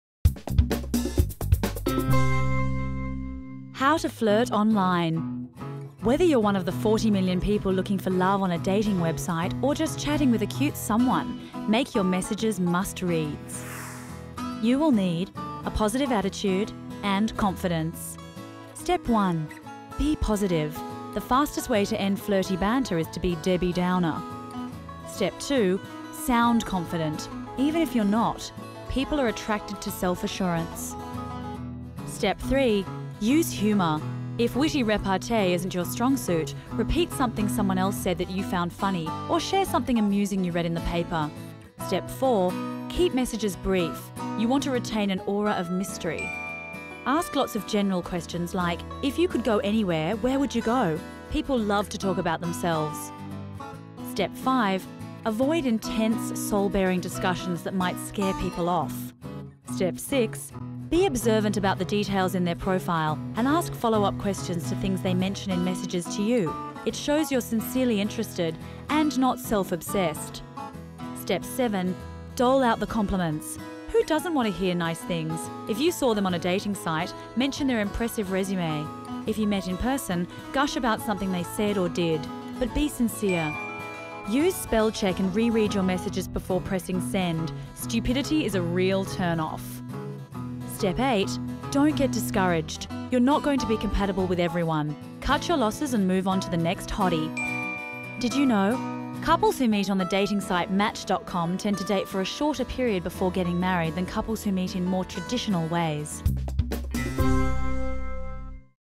How to Flirt Online
Whether you're one of the 40 million people looking for love on a dating website or just chatting with a cute someone, make your messages must-reads!

Step 1: Be positive
Be positive. The fastest way to end flirty banter is to be Debbie Downer.

Step 2: Be confident
Sound confident--even if you're not. People are attracted to self-assurance.

Step 3: Be funny
Use humor. If witty repartee isn't your strong suit, repeat something someone else said that you found funny, or share something amusing you read in the paper.

Step 4: Be brief
Keep messages brief--you want to retain an aura of mystery.

Tip
Ask lots of general questions like, "If you could go anywhere, where would you go?"--people love to talk about themselves.

Step 5: Avoid baring your soul
Avoid intense, soul-baring discussions that might scare people off.

Step 6: Be observant
Be observant about the details in their profile, and ask follow-up questions to things they mention in messages to you. It shows you're sincerely interested--and not self-obsessed.

Step 7: Give compliments
Dole out the compliments. Who doesn't want to hear nice things? If you saw them on a dating site, mention their impressive resume. If you met in person, gush about something they said or did. But be sincere.

Tip
Use spell-check and re-read your message before pressing "send"--stupidity is a real turnoff!

Step 8: Don't get discouraged
Don't get discouraged. You're not going to be compatible with everyone. Cut your losses and move on to the next hottie.

Couples who meet on the dating site Match.com tend to date for a shorter period before getting married than couples who meet in more traditional ways.